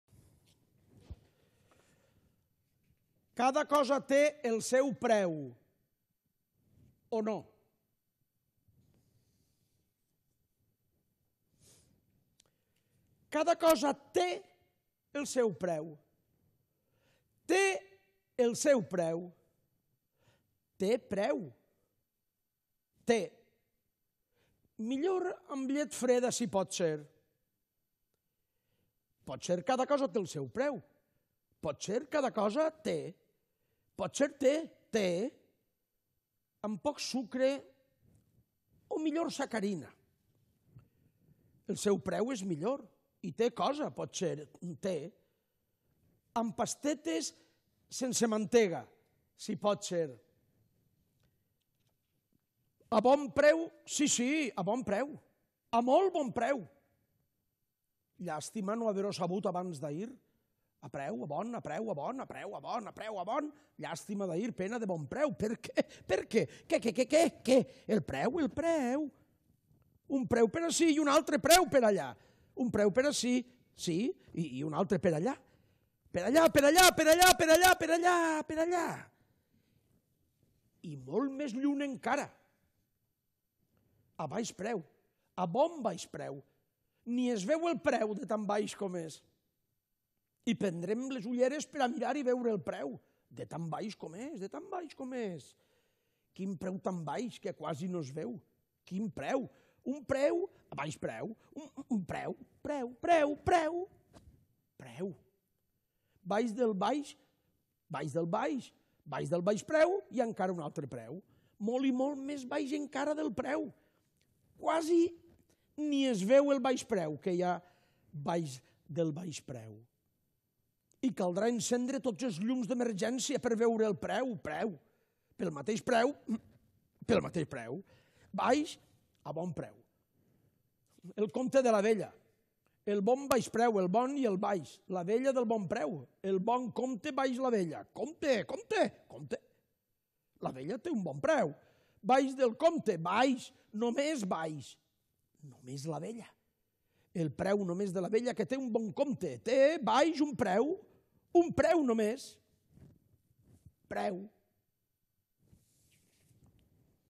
Cada cosa te el seu preu, o no?
De fil de vint (performances poètiques nº2)

AUTOR: JOSEP SOU
SOBRE EL AUTOR : ALCOI, 1951. Estudia filosofía i lletres a Valéncia i salamanca. Doctor en Belles Arts per la Universitat Miguel Hernández d'Elx. Professor de Pintura Urbana en la Universitat Miguel Hernández d'Elx.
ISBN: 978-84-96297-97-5
EDITADO POR : Editorial Electrónica de la Universidad Miguel Hernández.